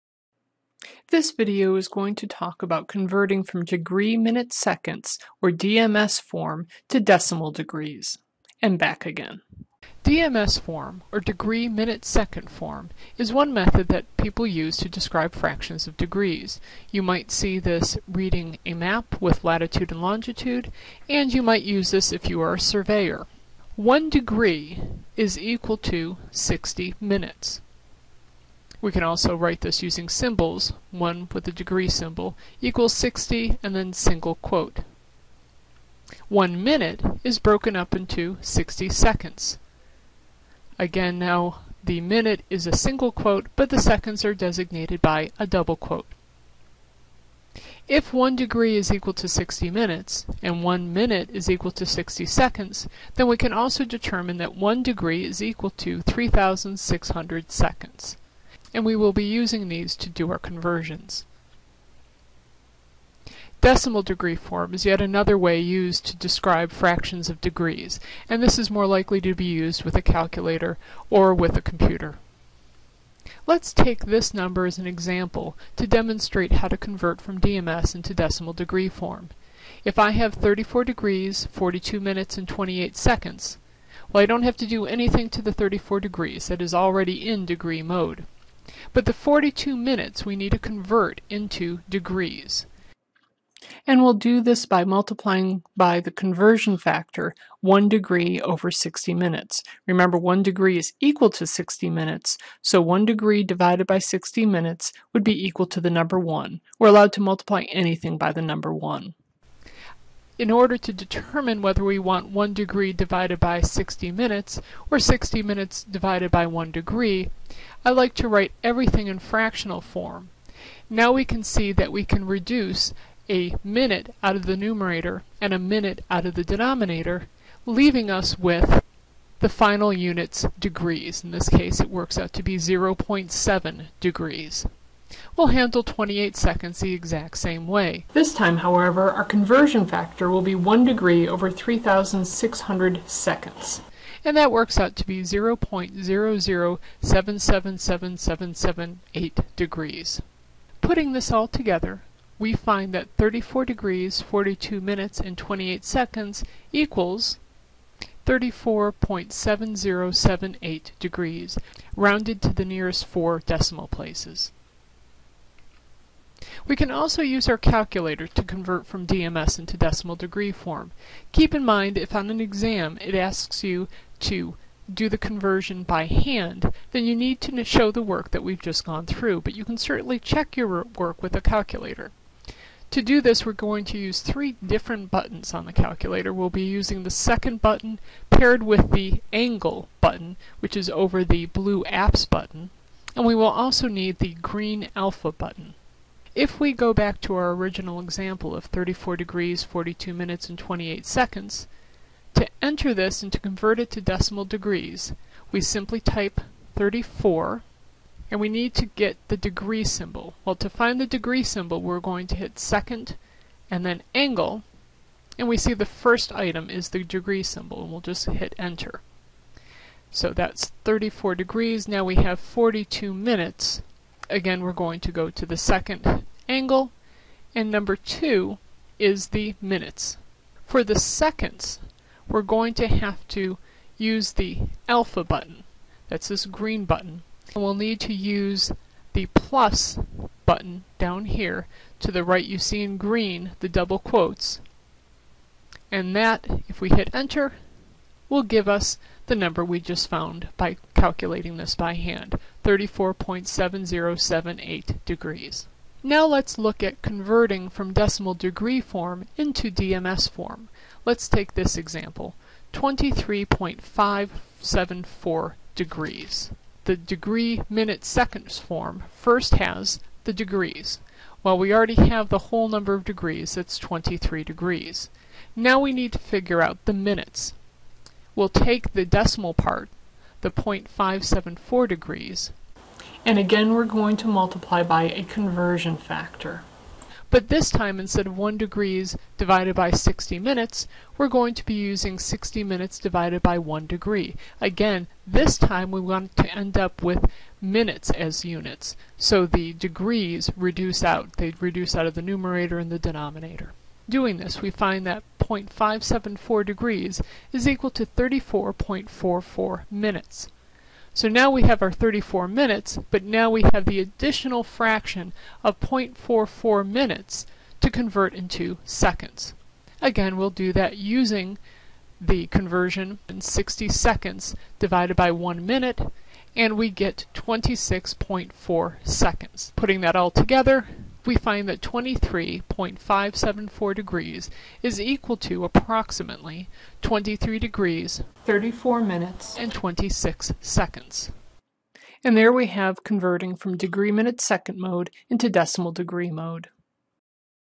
DMS to DD Form for Angles
Converting from DMS to decimal degree form of angle measure